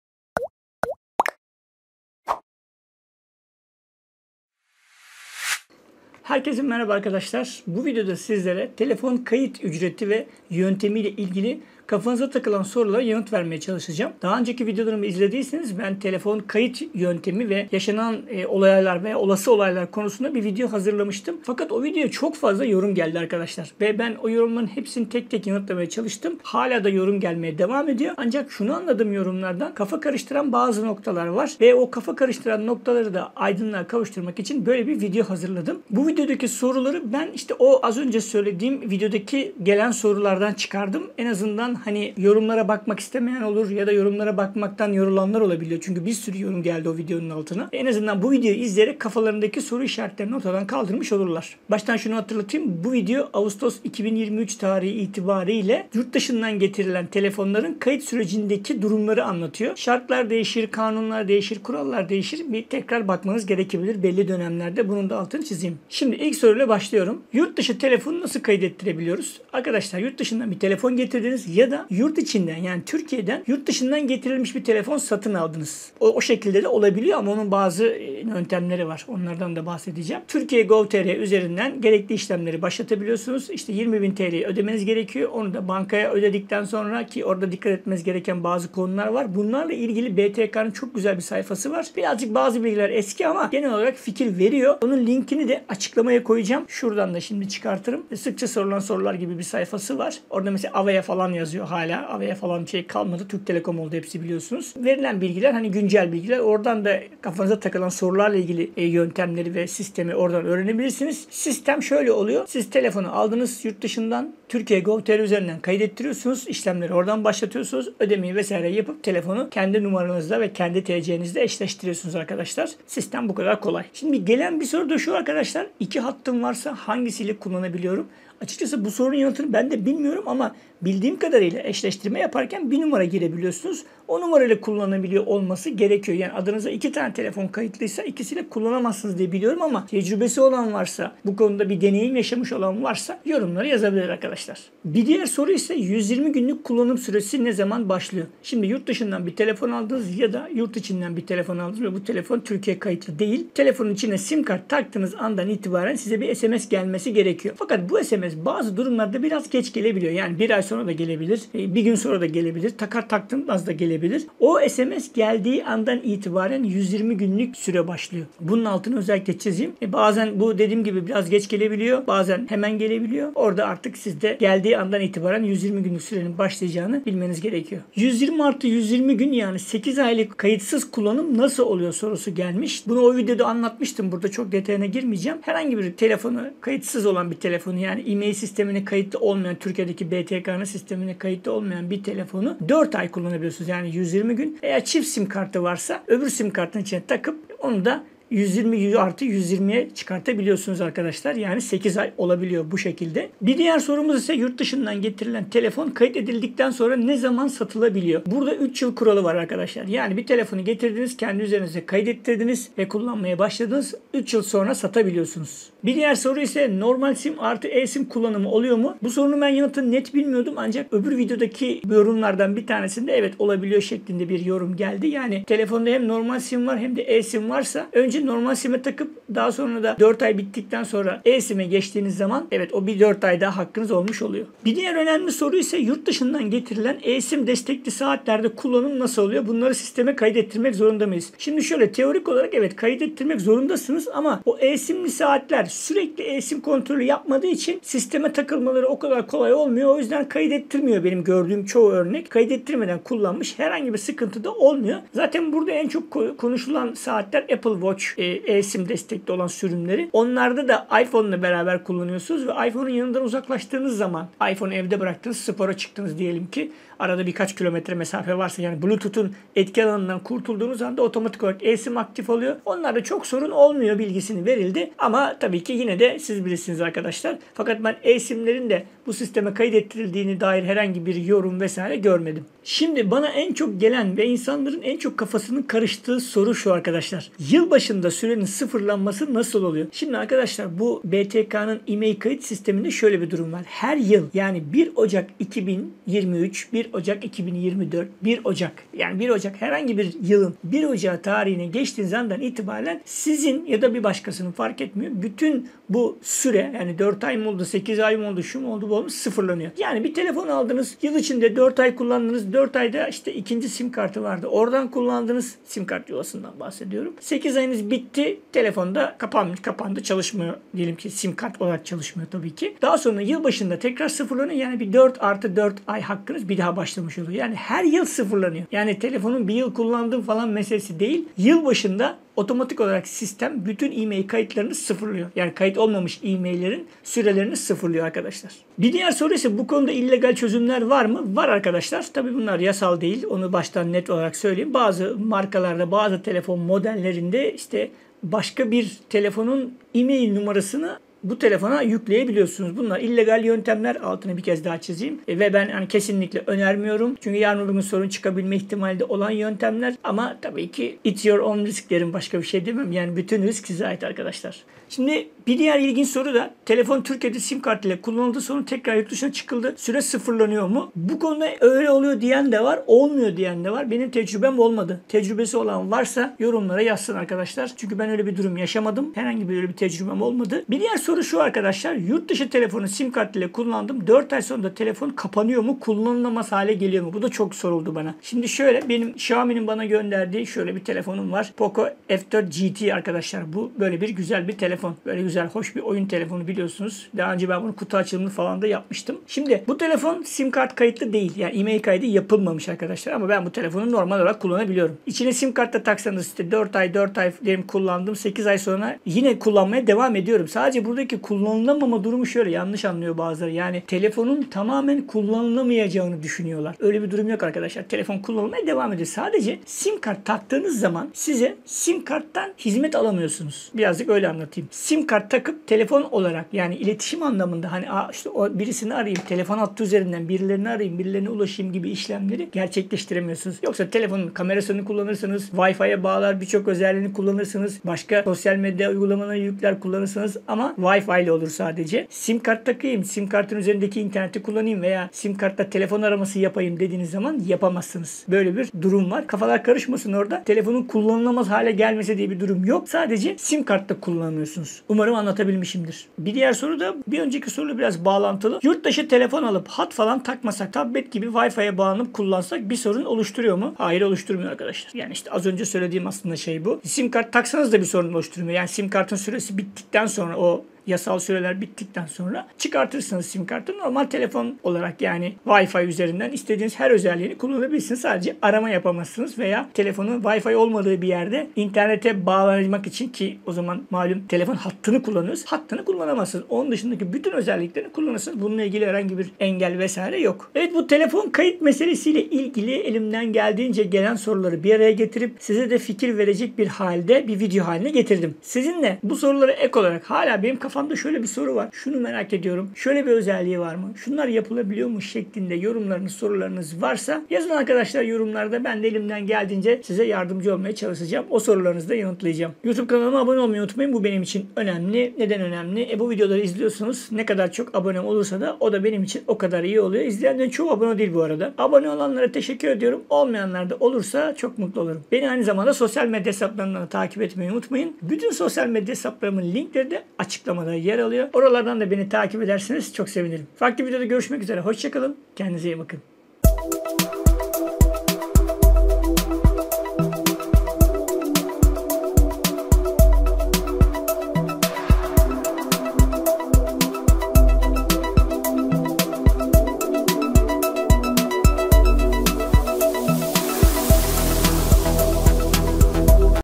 Yurtdışı telefon kayıt ücreti ve diğer kayıt sorularının yanıtları!
Telefon kayıt konusu ile ilgili daha önce sizlerle bir video paylaşmıştım. Bunun üzerine birçok soru geldi. Bu sorulardan seçtiklerimi bu videoda yanıtlıyorum. Ben önemli gördüğüm ve çok sorulan soruları seçmeye çalıştım. Buna rağmen sorularınız varsa yorumlara yazmaktan çekinmeyin.

Zaman çizelgesi

00:00 Açılış
00:30 Neden bu videoyu hazırladım?
01:07 Yurtdışı telefonu nasıl kayıt ettirebiliyoruz?
02:04 İki hattım varsa hangisiyle kullanabiliyorum?
02:25 120 gün kullanım süresi ne zaman başlıyor?
02:59 120 +120 gün (yani 8 ay) kayıtsız kullanım nasıl oluyor?
03:23 Yurtdışından getirilen telefon kayıt edildikten sonra ne zaman satılabiliyor?
03:36 Normal SIM + e-SIM kullanımı oluyor mu?
03:54 Yurtdışından getirilen e-SIM destekli saatlerde kullanım nasıl oluyor?
04:47 Yılbaşında sürenin sıfırlanması nasıl oluyor?
05:39 Bu konuda illegal çözümler var mı?
06:05 Telefon Türkiye'de SIM kart ile kullanıldı. Sonra tekrar yurtdışına çıkıldı. Süre sıfırlanıyor mu?
06:23 Yurtdışı telefonu SIM kart ile kullandım. 4 ay sonunda telefon kapanıyor mu? Kullanılamaz hale mi geliyor?
07:38 Yurtdışı telefon alıp  hat falan takmasak tablet gibi  wifi bağlanıp kullansak bir sorun  oluşturuyor mu?
08:11 Siz de soru sorabilirsiniz
08:33 Kanala abone olup beni takip edin
08:52 Kapanış